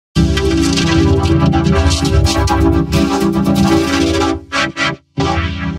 (REAL OR FAKE) An Audio Effect I'll Call SpectraYouTubePower
How To Make:

Rays: SpectraPower 2.0
Gradient Map:
Red
White
Red
White
Red
White
Red
White
Red

IL Vocodex: Power (Keys: YouTubeChorded) (Band Width 400%) Hello, It's Me! I Created This Video By Myself, Because It Was On My Mind And I Wanted To Create It.